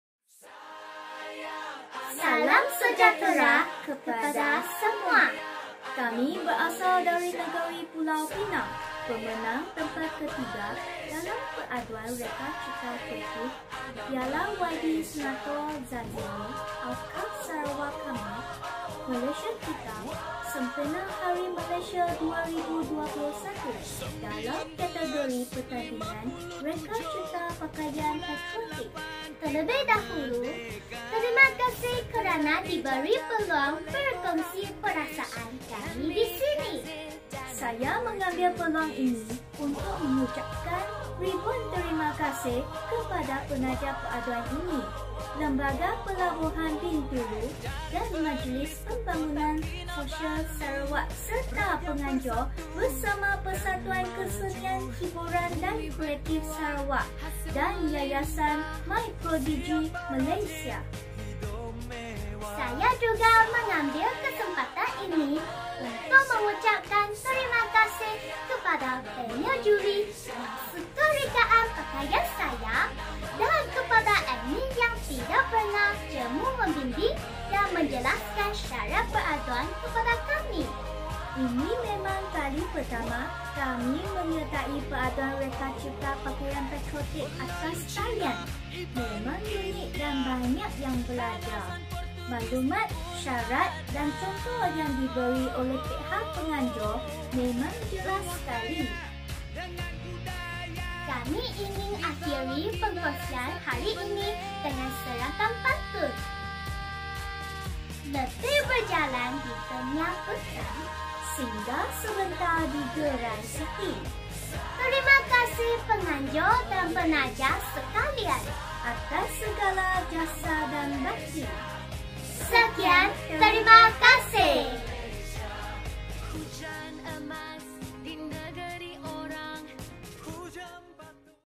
Puan Ng Siow Wai (Pulau Pinang) - Pemenang Tempat Ke3 (Kategori Pakaian Patriotik Kreatif)
Video ucapan Pemenang tempat ke3 Kategori Pakaian Patriotik Kreatif, Puan Ng Siow Wai bersama anak perempuannya.

Anjuran Bersama
PERSATUAN KESENIAN HIBURAN & KREATIF SARAWAK
YAYASAN MyPRODIGY MALAYSIA

Tajaan Bersama
LEMBAGA PELABUHAN BINTULU
MAJLIS PEMBANGUNAN SOSIAL SARAWAK